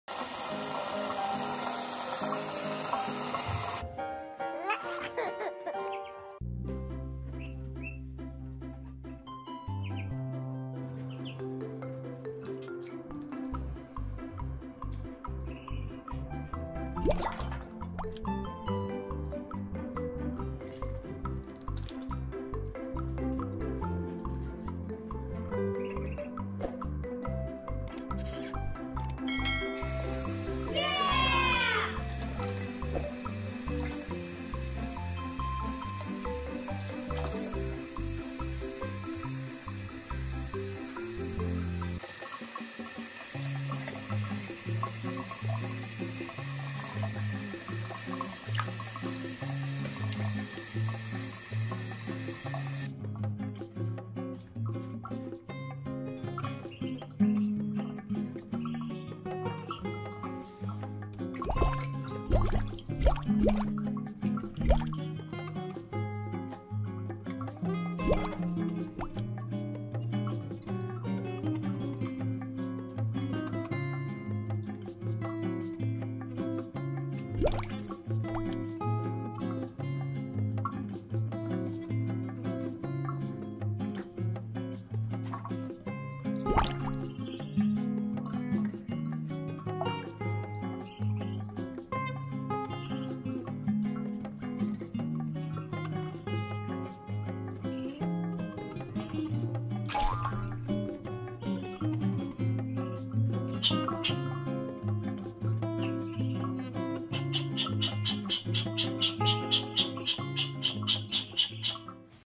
The Cutest Baby Ducks Swim in a Sink
Three little ducklings swim in a bathroom sink. They enjoy it so much!